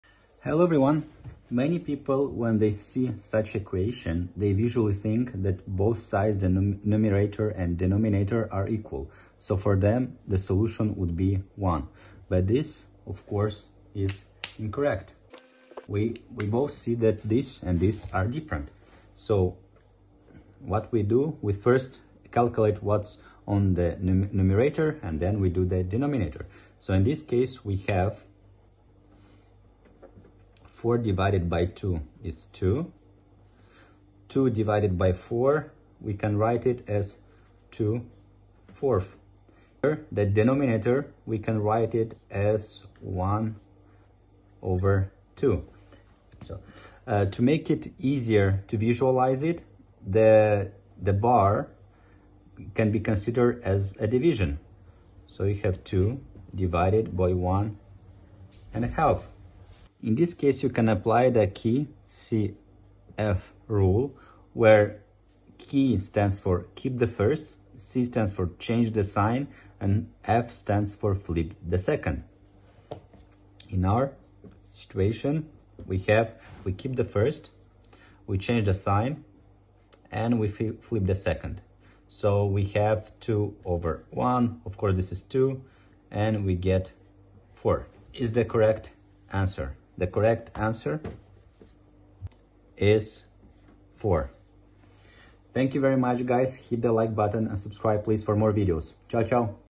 Solve This Tricky Math Problem | (4÷2)/(2÷4) = ?
Can you solve (4 ÷ 2) / (2 ÷ 4) correctly?

This tricky math problem confuses most people because of division inside parentheses and fractions. To get the right answer, you must apply both PEMDAS / BODMAS and the KCF rule (Keep–Change–Flip) for dividing fractions.

🔹 PEMDAS / BODMAS rules:

- P / B = Parentheses / Brackets
- E / O = Exponents / Orders (powers, roots)
- M & D = Multiplication & Division (from left to right)
- A & S = Addition & Subtraction (from left to right)

🔹 KCF Rule:

- Keep the first number
- Change the division sign to multiplication
- Flip the second fraction

👉 Most people get this one wrong. Can you get it right? The answer may surprise you!